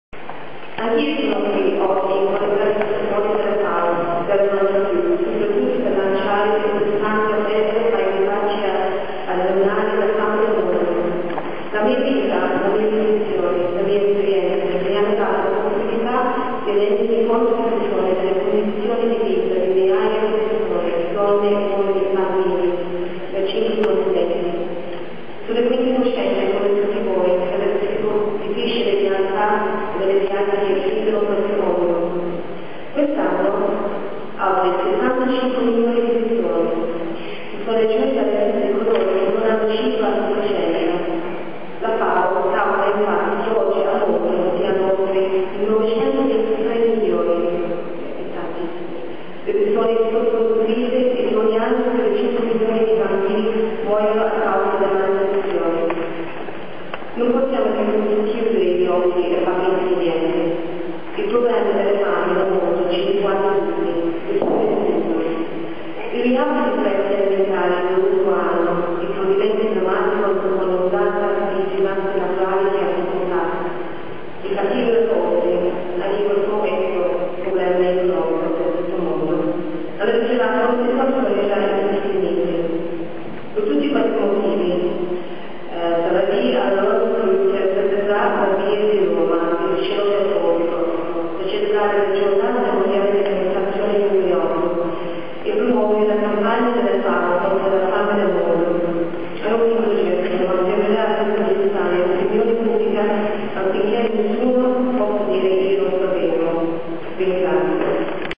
RUN FOR FOOD 2008 - FIONA MAY
Conferenza stampa di presentazione della terza edizione della Run For Food.